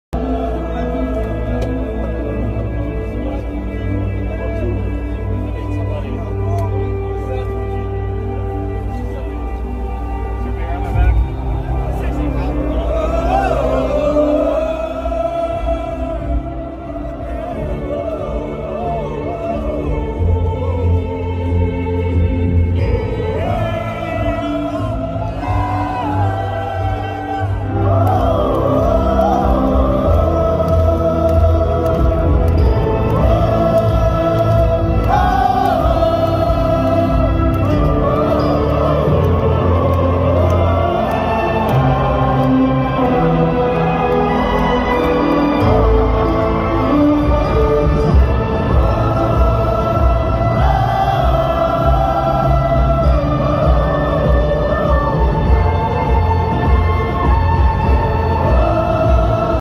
James Hetfield Feeling Great. Moments Before.....
taken from several shows and made into one Short.    James guitar Tech of 30? 40 years having a good laugh and interacting with the crowd.